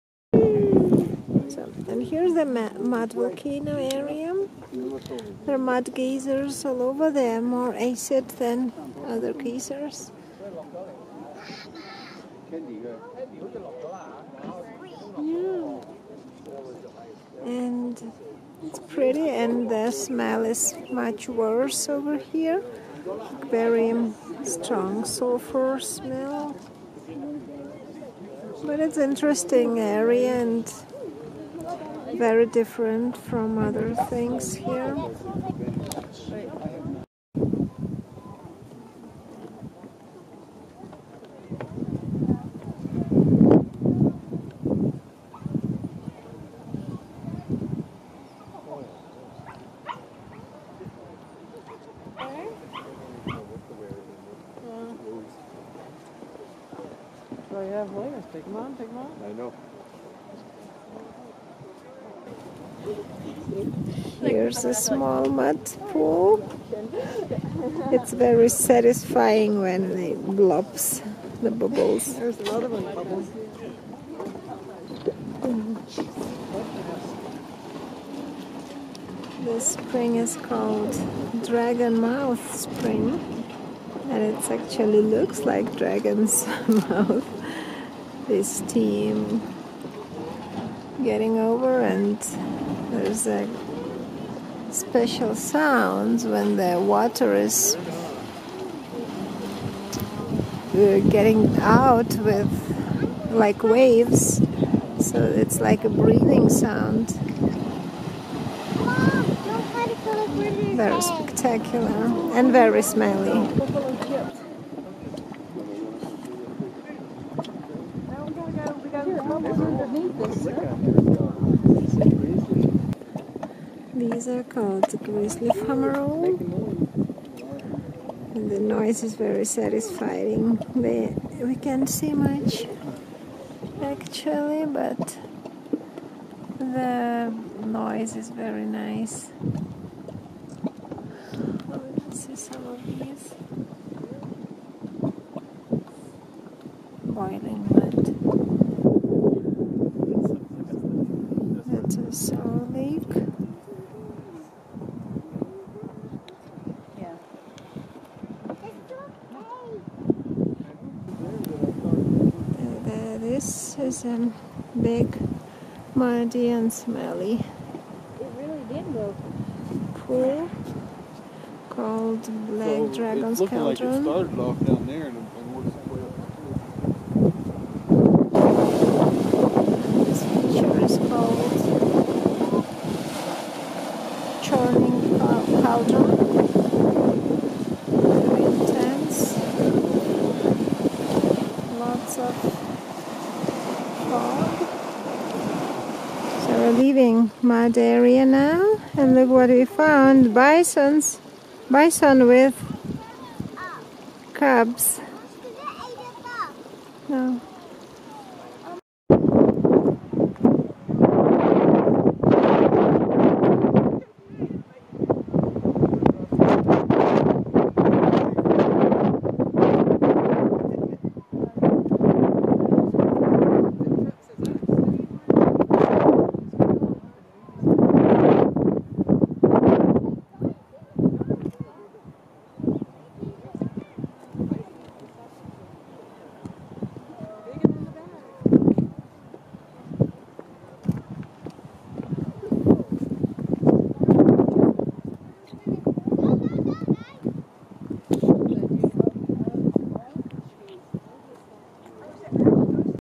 MUD VOLCANO, YELLOWSTONE NP
Yellowstone is probably one of the most incredible places on Earth. It's a place I always want to come back to. We've been there twice, and I can give you a little tip on when to visit. Probably the best time is the last week on August and the first week of September. The "Yellowstone season is really short". It can still snow and be cold and wet in May, and it can snow again in late September. This short period I've mentioned is perfect because of no more crowds and the weather is perfectly warm and nice.

Of course, you can visit it in summer, which is warm and nice too, but it's the busiest season. There are literally crowds of people, there are traffic jams, and it's exhausting (we did that too).

Mud volcano is an interesting park. Very acidic, very muddy and very smelly :)